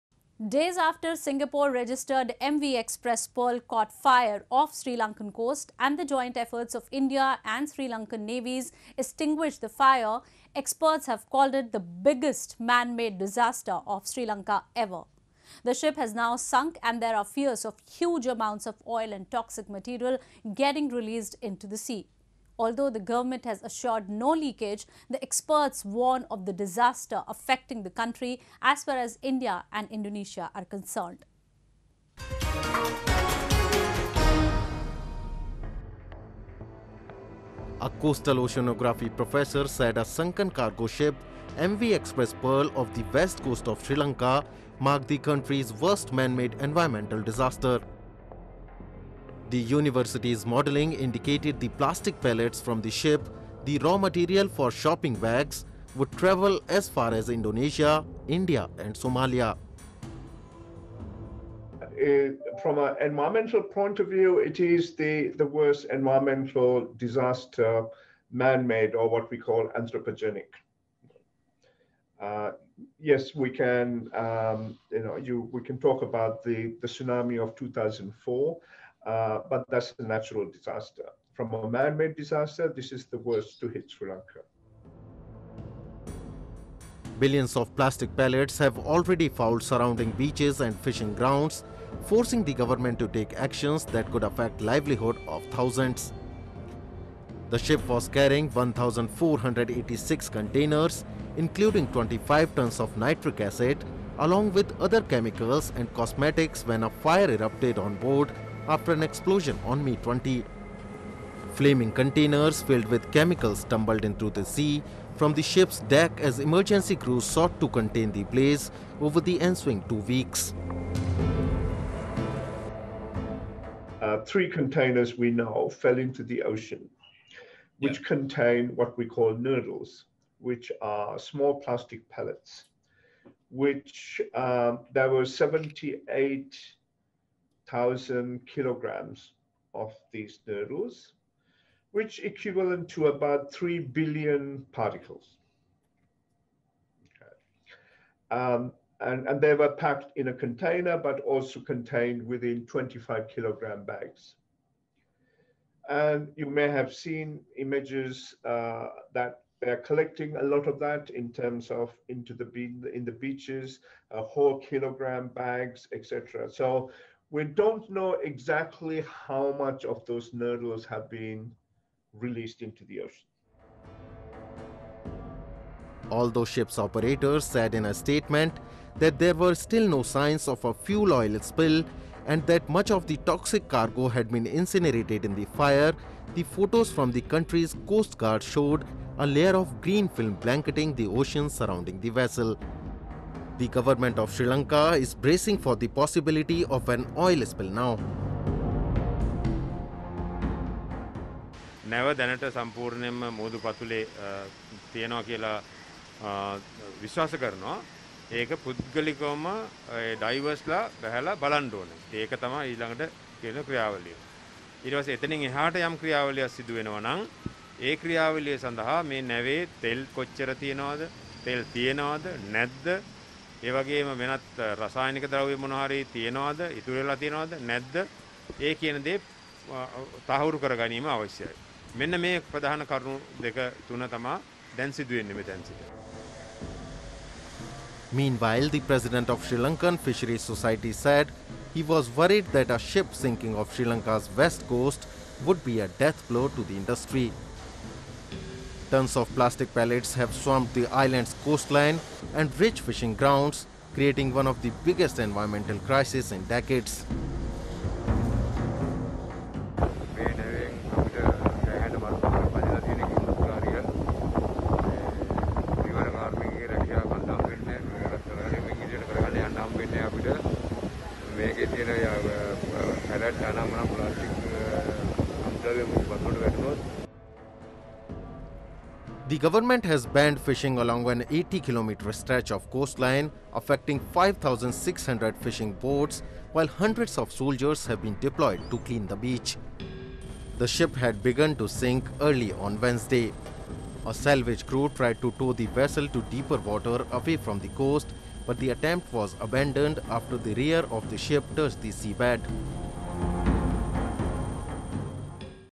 Ship sinks off Sri Lanka’s coast after ‘worst man-made disaster’
Days after Singapore-registered MV X-Press Pearl caught fire off Sri Lankan coast and the joint efforts of Indian and Sri Lankan navies extinguished the fire, experts have called it the biggest man-made disaster of Sri Lanka ever. The ship has now sunk and there are fears of huge amounts of oil and toxic material getting released into the sea. Although the government has assured no leakage, the experts warn of the disaster affecting the country as far as India and Indonesia.